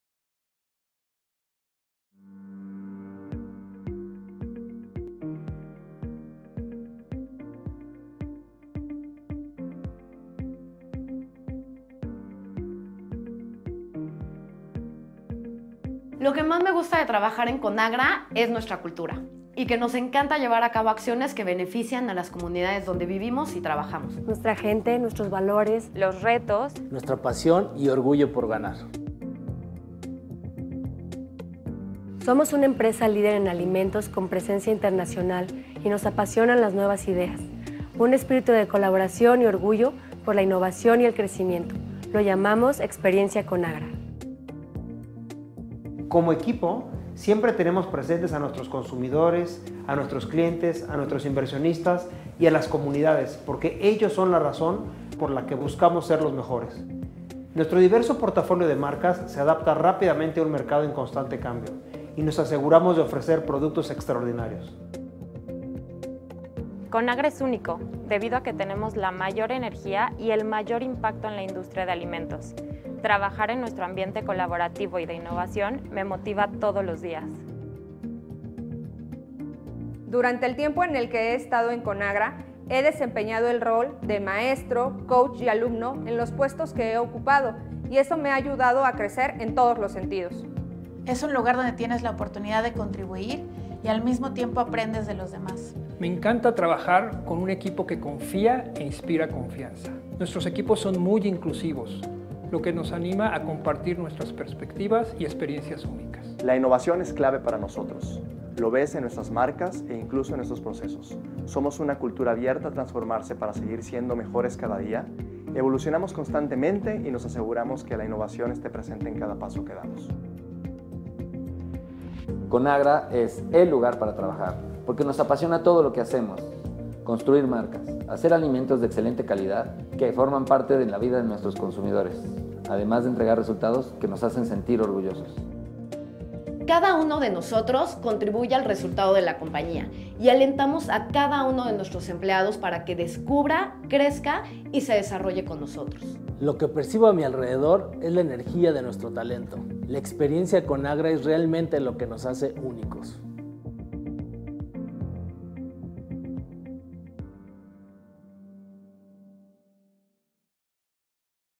The Conagra Experience: Mexico
Conagra Brands en México tiene su sede en la alcaldía Miguel Hidalgo, en la Ciudad de México, y cuenta con más de 150 colaboradores(as).

Las plantas e instalaciones de Conagra Brands en nuestro país están ubicadas en Irapuato, Guanajuato, y emplean a más de 500 personas. La ciudad esta a cuatro horas en automóvil de la Ciudad de México y se encuentra en una posición estratégica para los centros de distribución regionales. Irapuato es actualmente una región con un crecimiento económico importante y es conocida por su producción de frutas y verduras.

Conagra Brands in Mexico is headquartered in Mexico City, the ninth most populated urban area in the world and the second most populated in North America. Over 150 employees work for Conagra Brands in Mexico City, the capital and most important political, cultural, educational and financial center in the country.

Conagra Brands in Mexico's plant facilities are located in Irapuato, Guanajuato, and employ more than 500 people. The city is a four-hour drive from Mexico City and is located in a strategic position for regional distribution centers. Irapuato is currently a region with an important economic growth and well-known for its production of vegetables and fruits.